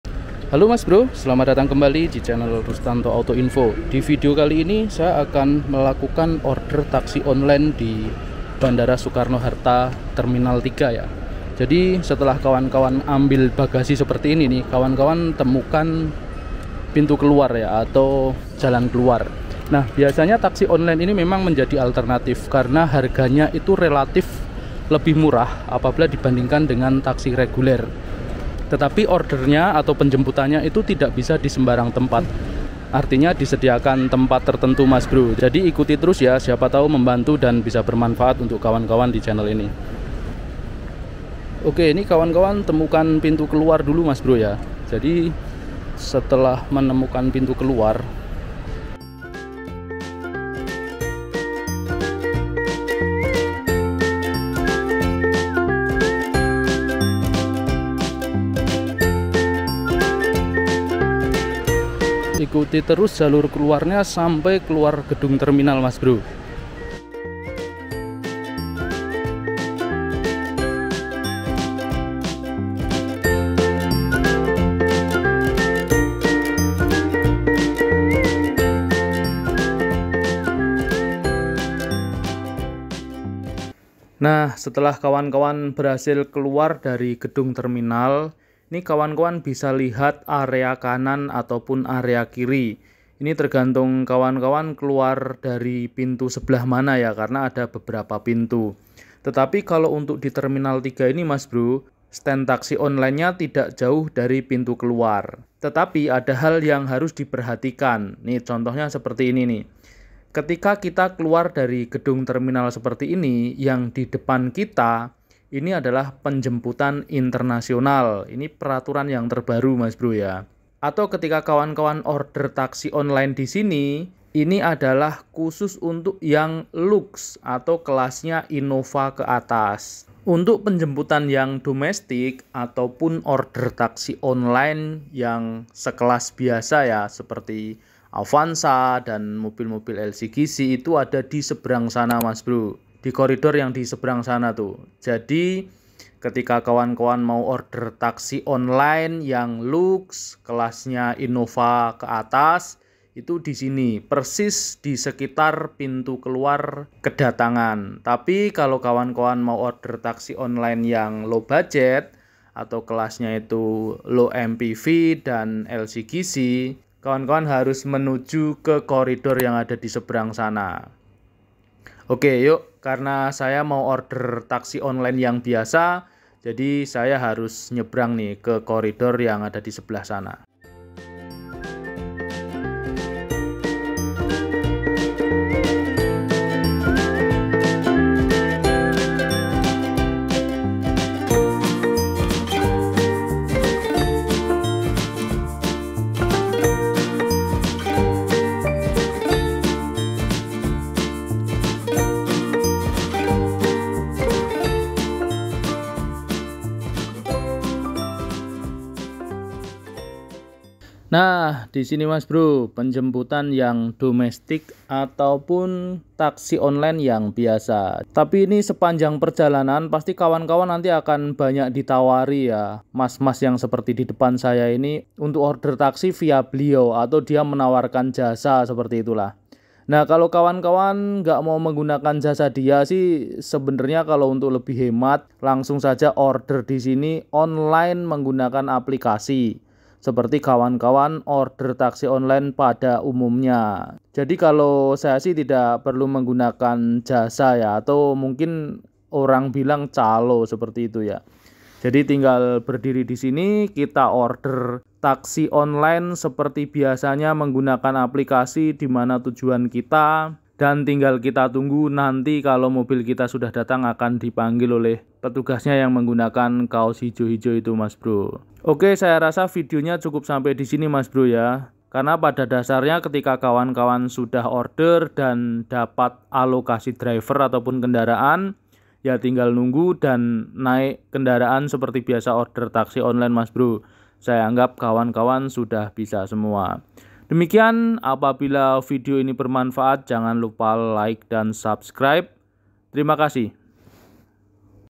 Cara Order Taksi Online di Terminal 3 Soekarno Hatta - Terbaru September 2023
Halo semuanya divideo kali ini saya mendokumantasikan proses saya order taksi online di terminal 3 bandara soekarno hatta. video ini saya ambil tanggal 21 September 2023. Semoga bermanfaat